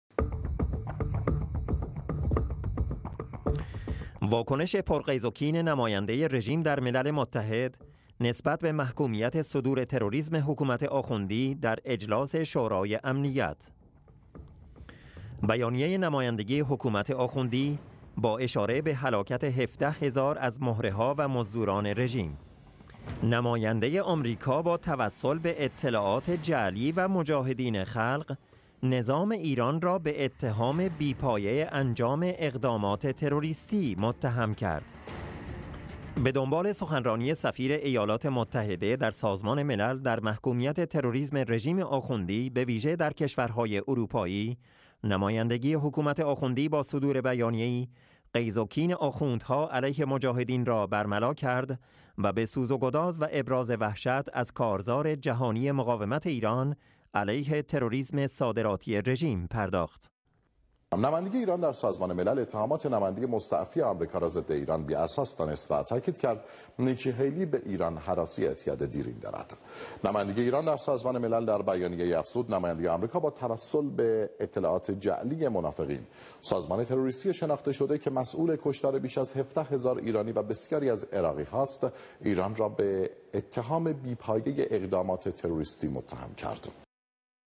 نمایندگی رژیم ایران: نیکی هیلی با توسل به اطلاعات مجاهدین خلق ایران را متهم کرد
واكنش پر غيظ و كين نماينده رژيم در ملل متحد نسبت به محكوميت صدور تروريسم حكومت آخوندي در اجلاس شوراي امنيت

بیانیه نمايندگي حکومت آخوندی  با اشاره به هلاكت ۱۷ هزار از مهره‌ها و مزدوران رژيم:
نمايندة آمريکا با توسل به اطلاعات جعلي و مجاهدين خلق
نظام ايران را به اتهام بي‌پايه انجام اقدامات تروريستي متهم کرد

به‌دنبال سخنراني سفير ايالات متحده در سازمان ملل،در محكوميت تروريسم رژیم آخوندي به ویژه در كشورهاي اروپايي، نمايندگي حکومت آخوندی با صدور بيانيه‌يي غیظ و کین آخوندها علیه مجاهدین را برملاکرد و به سوز و گداز و ابراز وحشت از كارزار جهاني مقاومت ایران علیه تروريسم صادراتی رژيم پرداخت:
منبع:ايسنا ۹۷/۸/۲۸

ايران هراسي اعتياد ديرينه نماينده دائم آمريكا در سازمان ملل است

تلويزيون رژيم - ۲۹ آبان ۱۳۹۷

 نمايندگي ايران در سازمان ملل اتهامات نماينده مستعفي آمريکا را ضد ايران بي اساس دانست و تأکيد کرد  نيکي هيلي به ايران هراسي اعتياد ديرين دارد.

 نمايندگي ايران در سازمان ملل در بيانيه‌اي افزود نماينده آمريكا با توسل به اطلاعات جعلي منافقين سازمان تروريستي شناخته شده که مسول کشتار بيش از ۱۷هزار ايراني و بسياري ازعراقي ها است ايران را به اتهام بي پايه اقدامات تروريستي متهم کرد.

منبع: تلويزيون رژيم (شبكة خبر): ۹۷۰۸۲۹